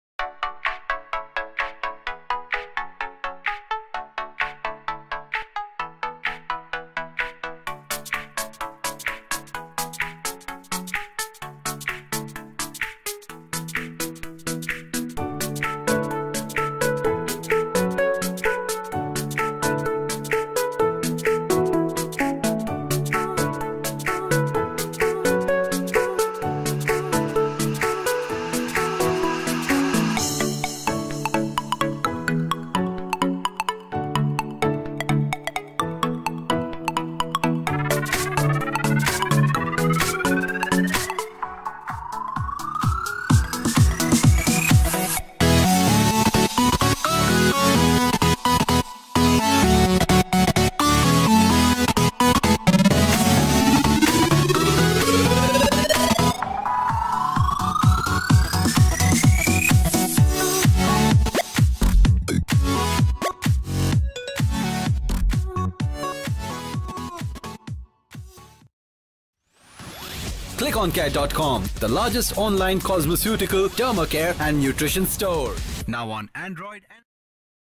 Ketriplin C Sprain Cream On ClickOnCare
Ketriplin C Cream  is used to relieve pain associated with recent muscle or joint injuries such as sprains, strains, or sports injuries. It also relieves pain from rheumatoid arthritis etc.